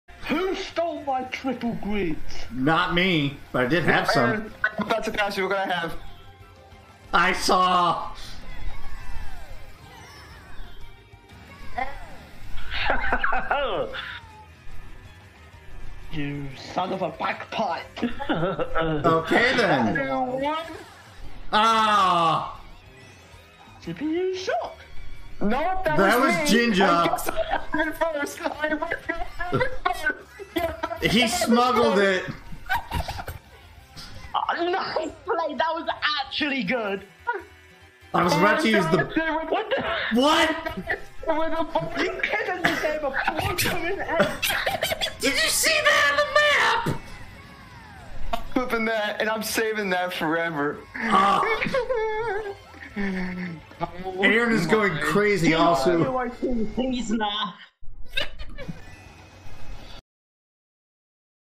AaronTSGamer - Smart Play by Ginja
- Watch the King Boo on the minimap to understand the context of this short. This was actually pretty funny.